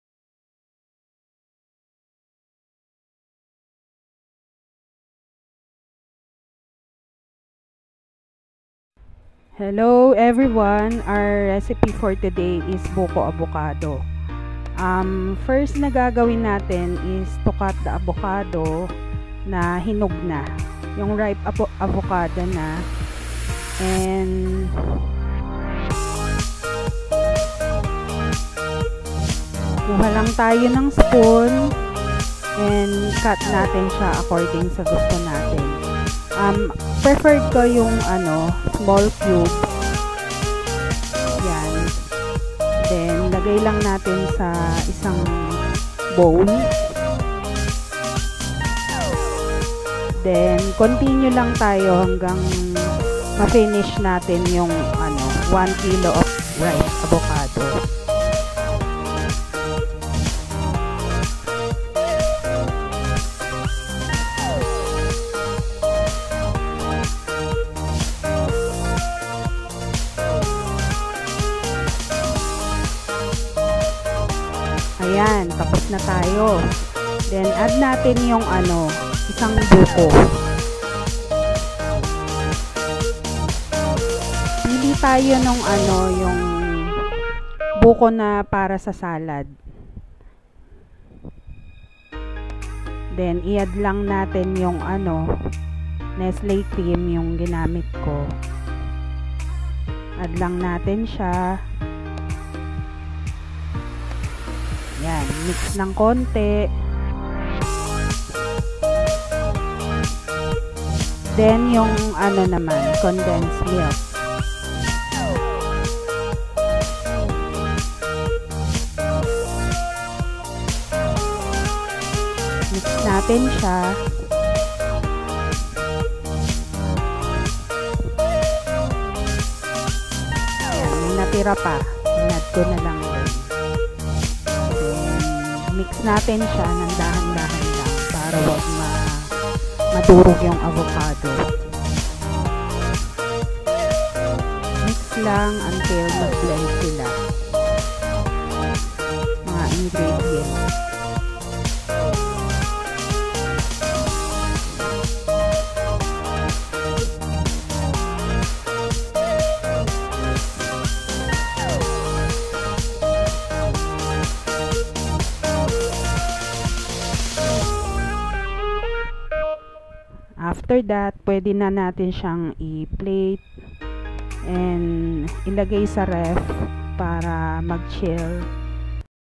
BUKO AVOCADO | BUKO AVOCADO CREAMY SALAD | 4 Ingredients Salad Ni Ninang Julie
Buko is an all season fruit, bountifully harvested in our country, the Philippines...Avocado though seasonal is a very versatile fruit, when combined together, this two makes a perfect dessert!

Ingredients:

1 piece Buko (for salad, not soft not hard) shredded
1 Can Condensed Milk (300 ml / 390 grams) I used Alaska
1 pack All Purpose Cream (250 ml) I used Nestle Cream
1 kilo Avocado (washed, deseeded and cut into small squares

Procedure:

Wash all the Avocado, deseed and cut into small squares or watever shape/cut you wanted. Place them into a mixing bowl.
Put the shredded Buko after, then the Nestle Cream and Alaska condensed milk. Mix carefully/well. Chill it inside the ref for 2 or more hours, after transferring the  salad into a container with cover. Serve chilled, happy eating! 🙂